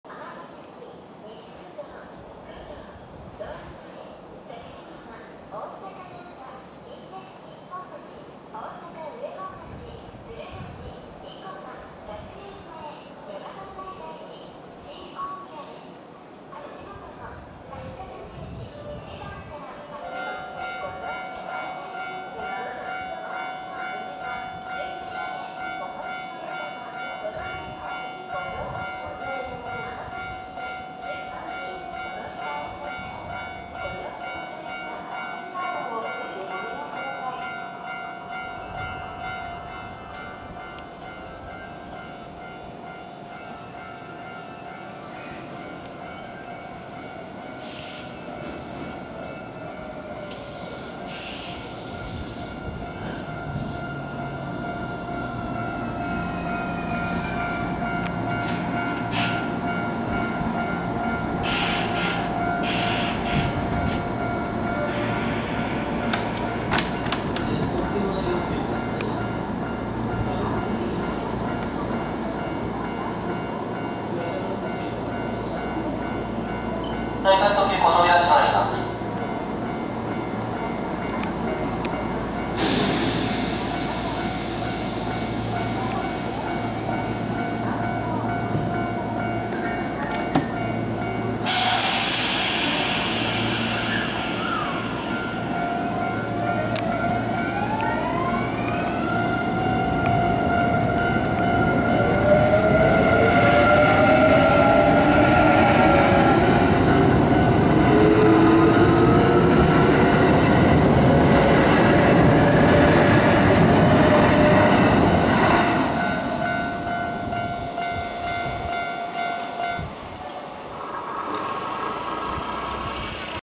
【近鉄車快急到着＆発車！】阪神電車 近鉄9020系＋9820系 近鉄奈良行き快速急行 武庫川駅
むこがわ